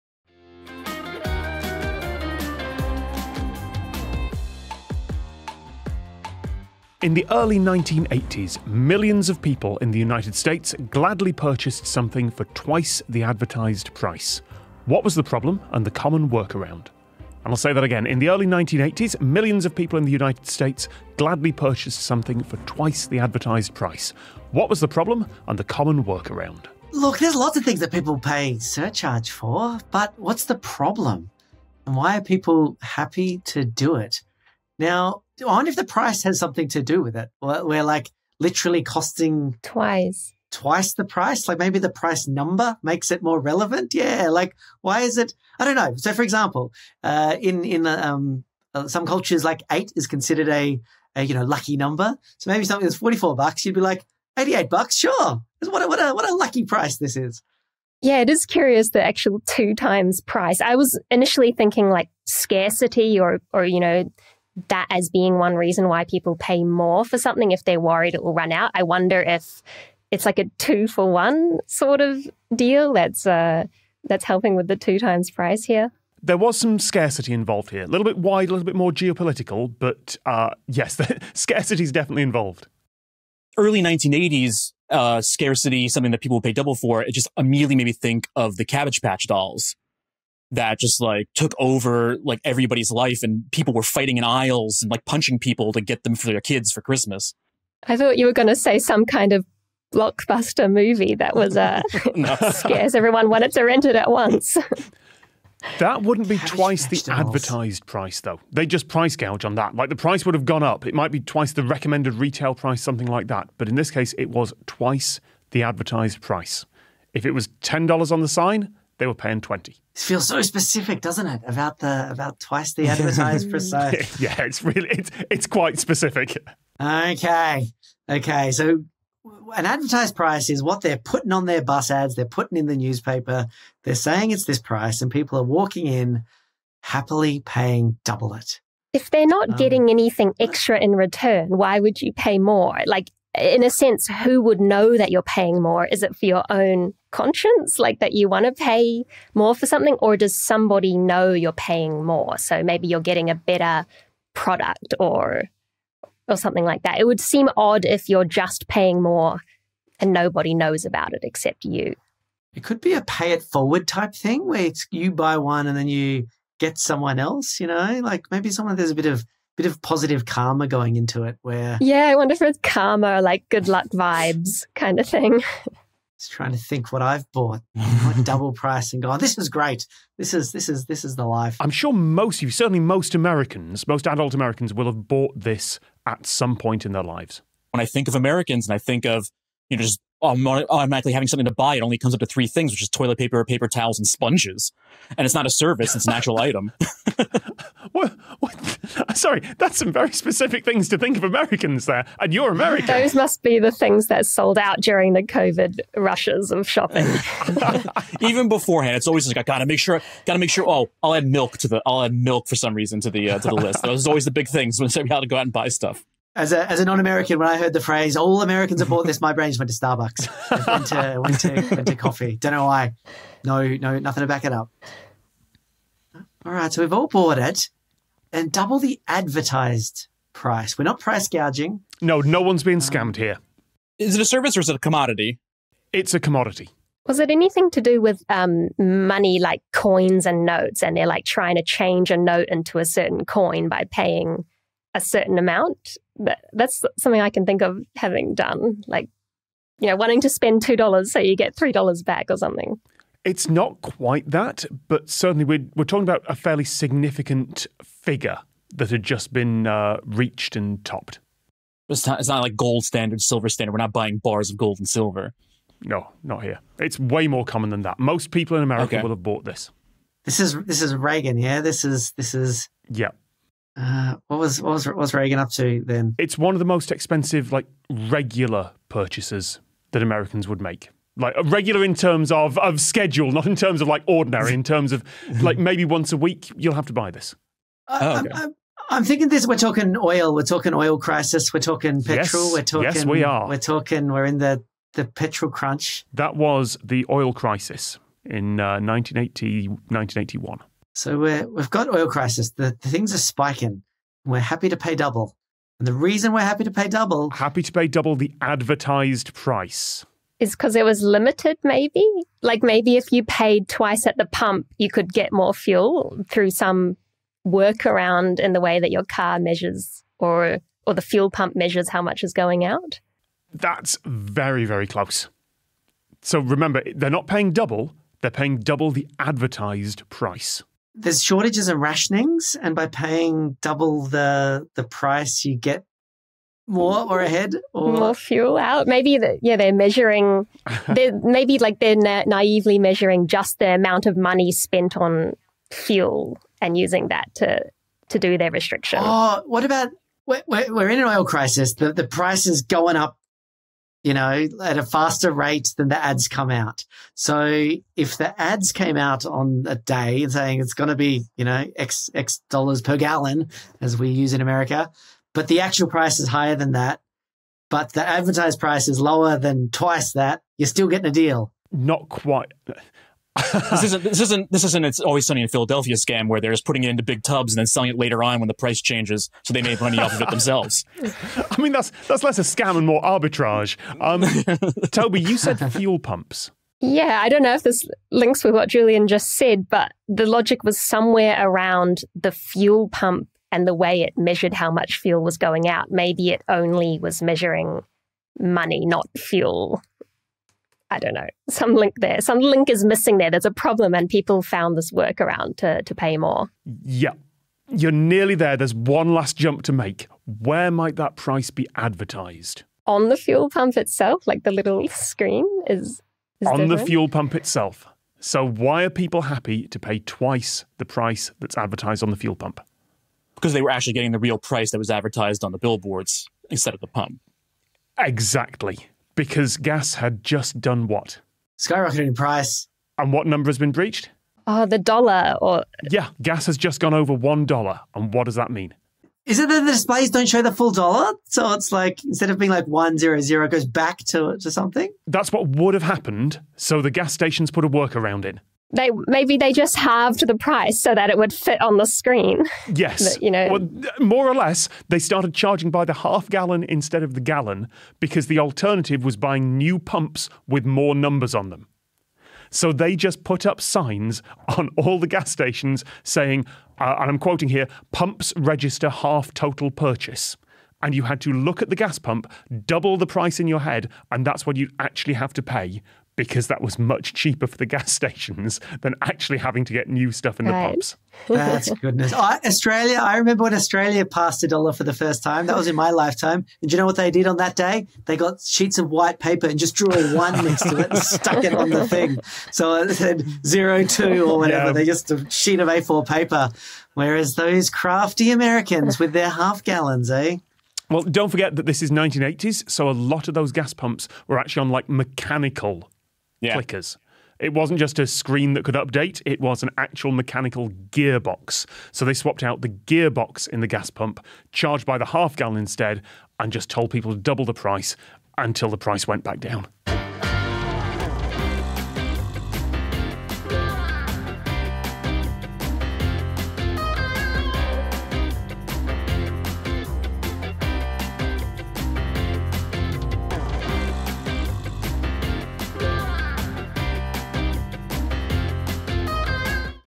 Gladly paying twice the price
Toby Hendy ('Tibees'), Matthew Schuchman ('Overdue Rentals') and Julian O'Shea face a question about a particular pricing problem.

QUESTION PRODUCER: David Bodycombe.

RECORDED AT: The Podcast Studios, Dublin.
EDITED BY: Julie Hassett.